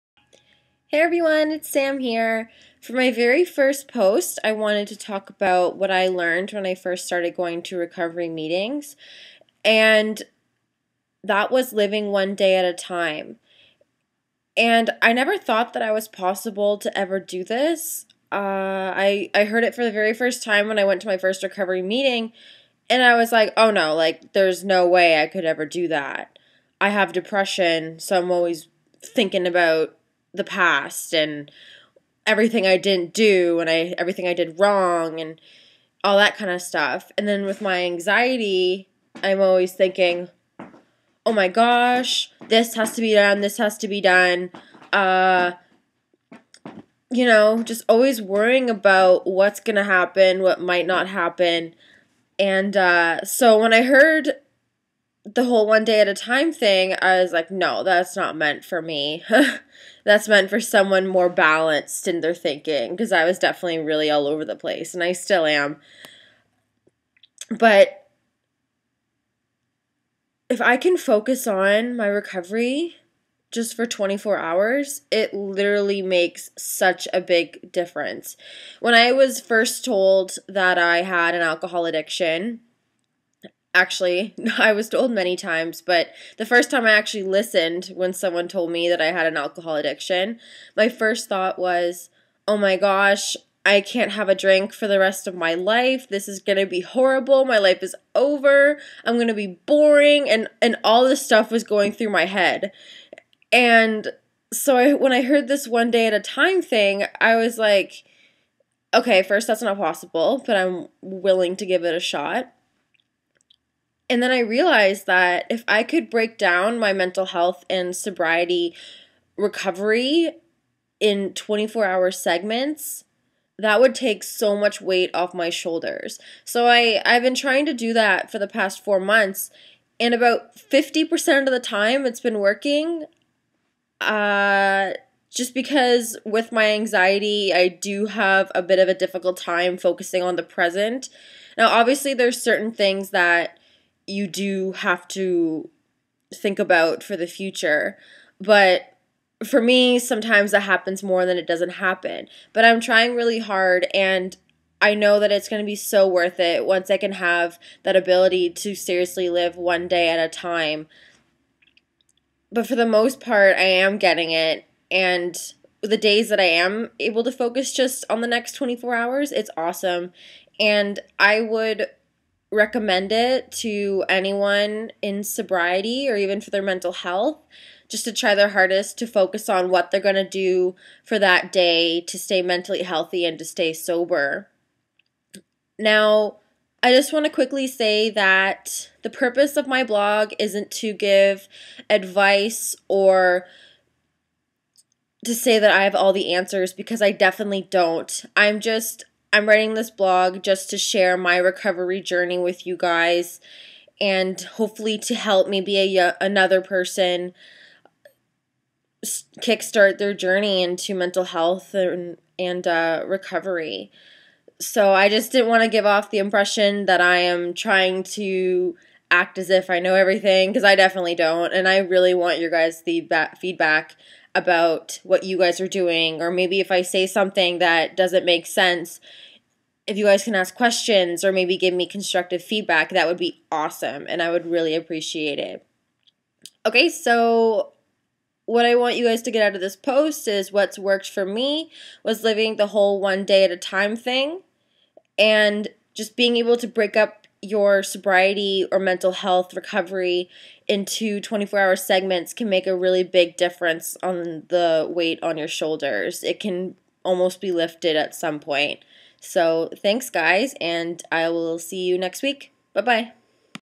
Living One Day at a Time in Substance Abuse Recovery
Living one day at a time is critical for successful substance abuse recovery. Samantha U'Ren explains why in Mental Health for a Digital Generation blog.

Substance Abuse Recovery Means Living One Day at a Time blog